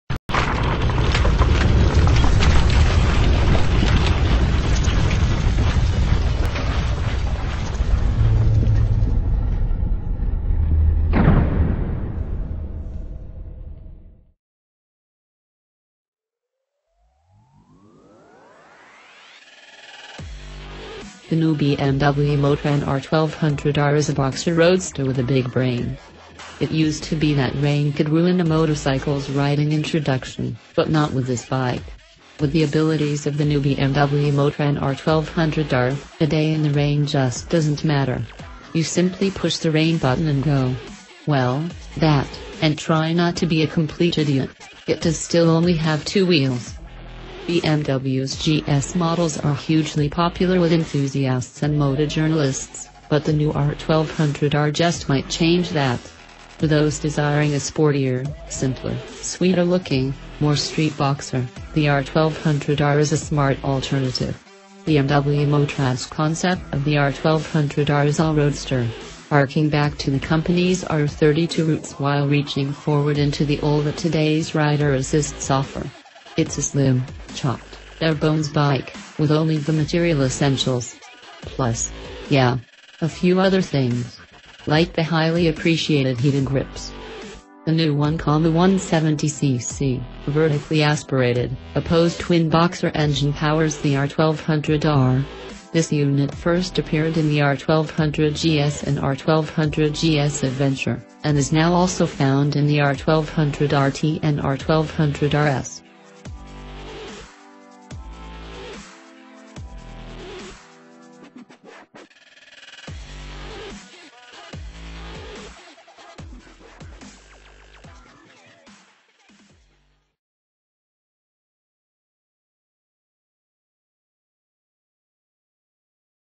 Otto Bike l 2019 BMW R1200R Naked Exclusive Features Edition First Impression HD
Otto Bike l 2019 BMW R1200R Naked Exclusive Features Edition First Impression  HD-The new BMW Motorrad R1200R is a boxer roadster with a big brain. It used to be that rain could ruin a motorcycle’s riding introduction, but not with this bike. With the abilities of the new BMW Motorrad R1200R, a day in the rain just doesn’t matter.

██████ GET DAILY UPDATES! NOW! ██████